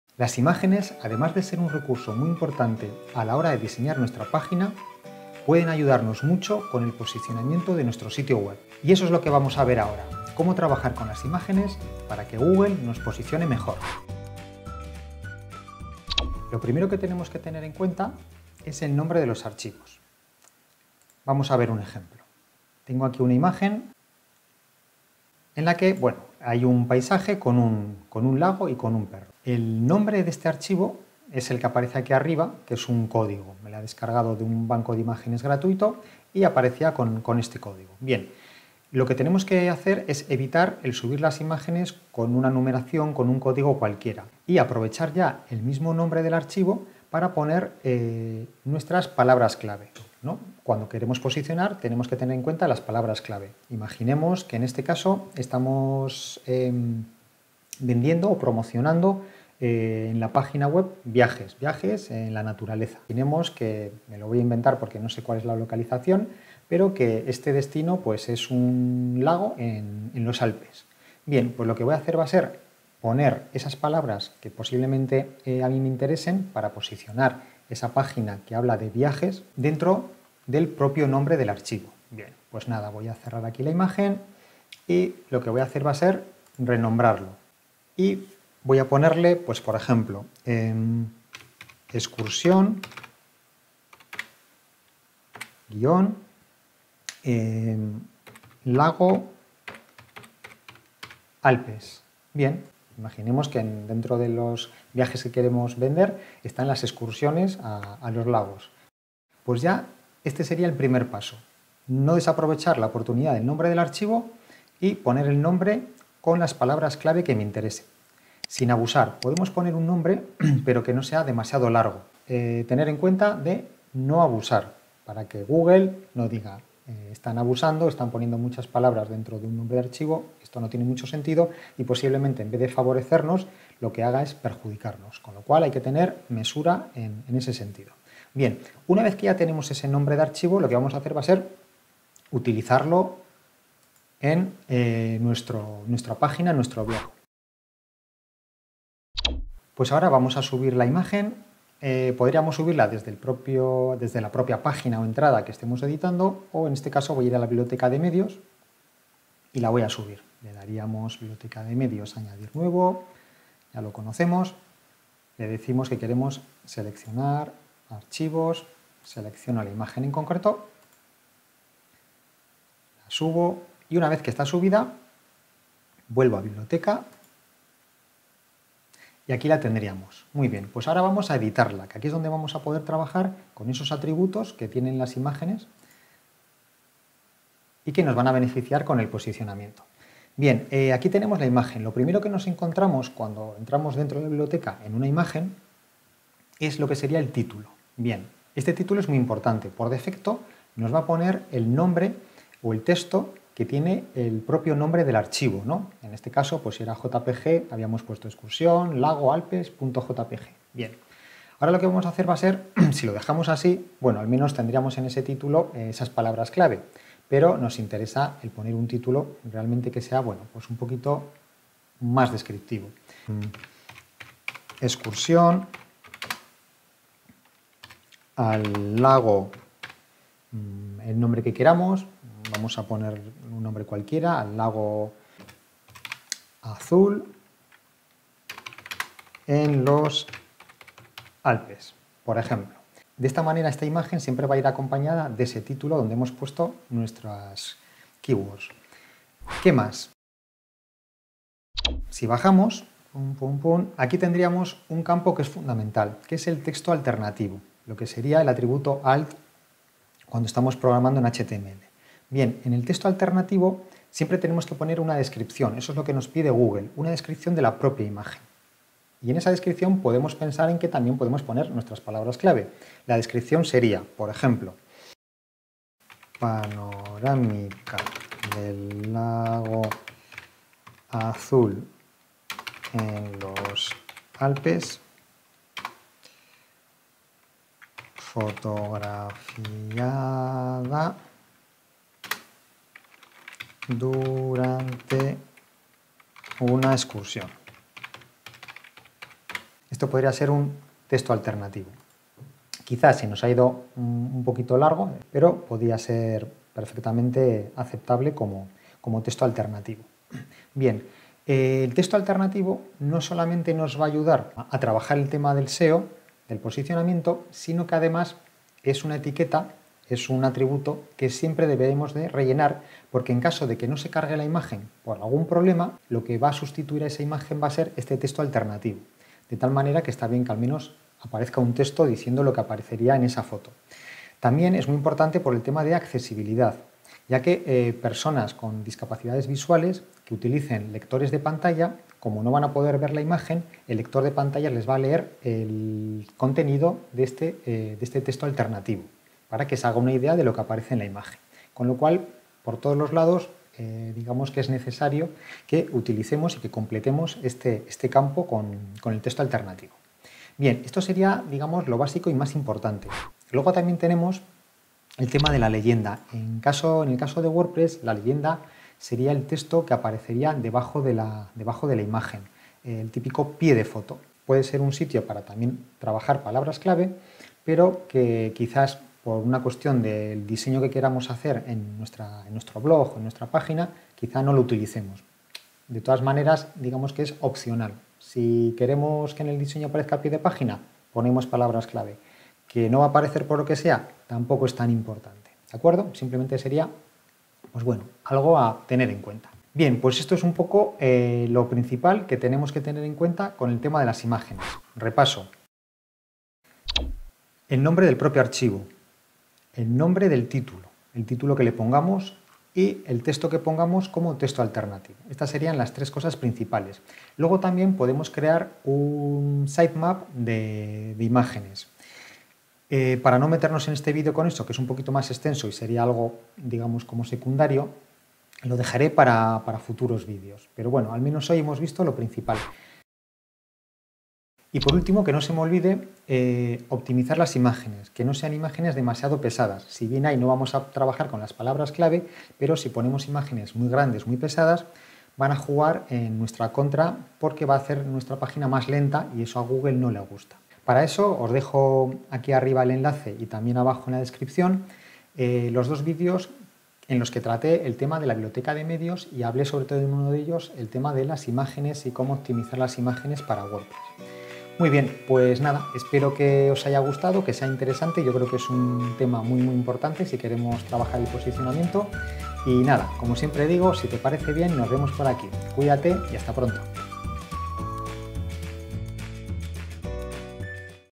Trabajar el SEO en las IMÁGENES en WordPress ⭐
Las IMÁGENES, nos pueden ayudar mucho con el POSICIONAMIENTO d nuestro SITIO WEB. Vamos a ver cómo trabajar con las imágenes y sus atributos para que Google nos posicione mejor.

En este tutorial en español de WordPress vamos a ver:
00:00 Presentación
00:14 1. El nombre de archivo
02:27 2. El atributo title
04:10 3. El texto alternativo
06:04 4. La leyenda
06:45 Repaso
07:13 Consejo para posicionar mejor